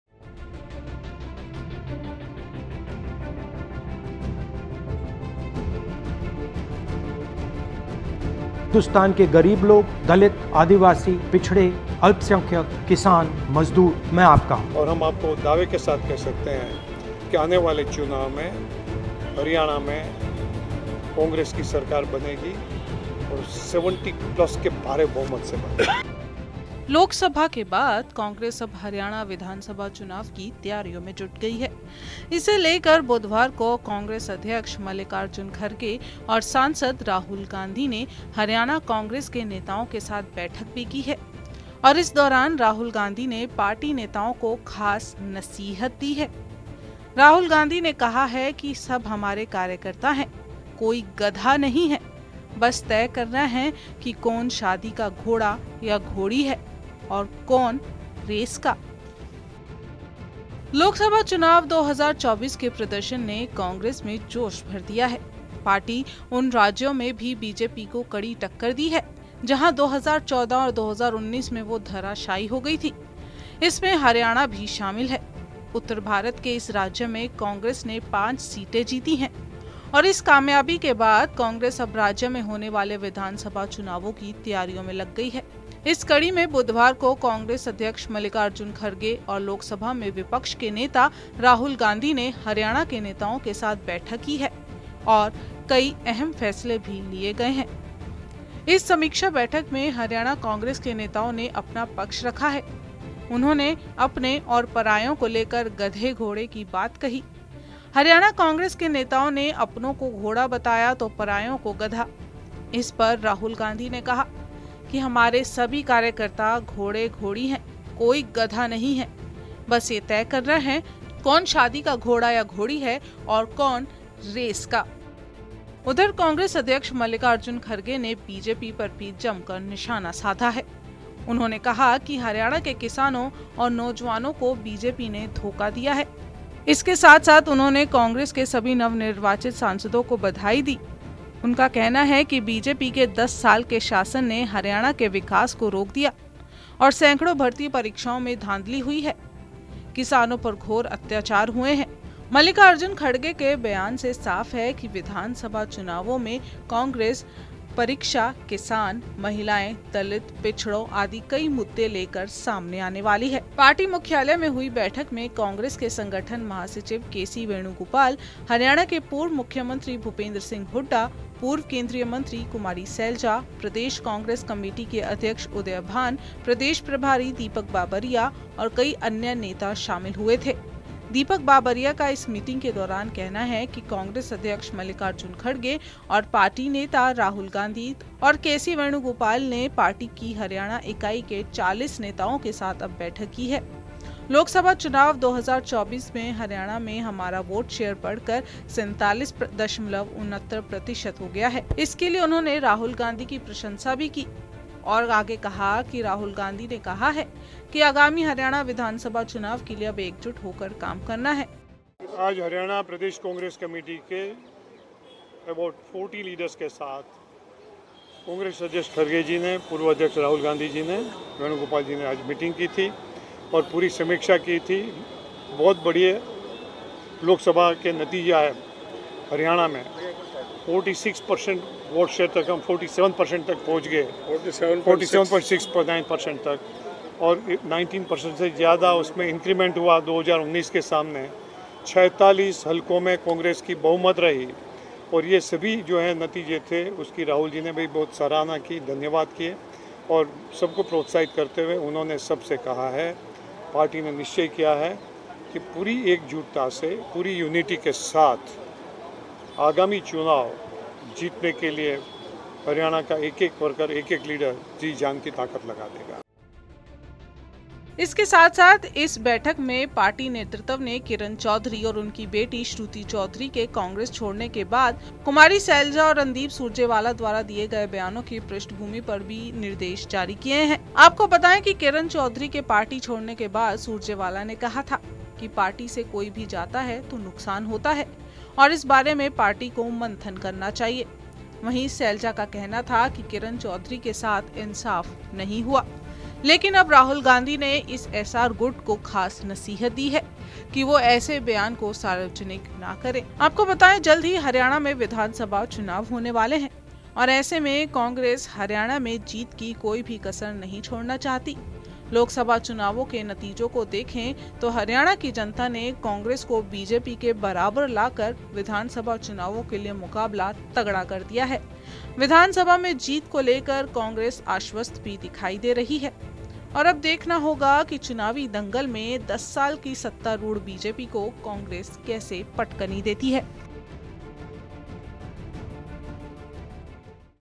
Haryana Assembly Election के लिए Rahul Gandhi ने की Congress के 40 नेताओं के साथ बैठक लेकिन...
लोकसभा चुनाव-2024 के प्रदर्शन ने कांग्रेस में जोश भर दिया है. पार्टी उन राज्यों में भी बीजेपी को कड़ी टक्कर दी जहां 2014 और 2019 में वो धाराशायी हो गई थी. इसमें हरियाणा भी शामिल है. उत्तर भारत के इस राज्य में कांग्रेस ने 5 सीटें जीती हैं. इस कामयाबी के बाद कांग्रेस अब राज्य में होने वाले विधानसभा चुनाव की तैयारियों में जुट गई है
The performance of Lok Sabha elections-2024 has filled the Congress with enthusiasm. The party gave a tough competition to BJP even in those states where it had failed in 2014 and 2019. Haryana is also included in this. Congress has won 5 seats in this state of North India. After this success, Congress has now started preparations for the assembly elections to be held in the state.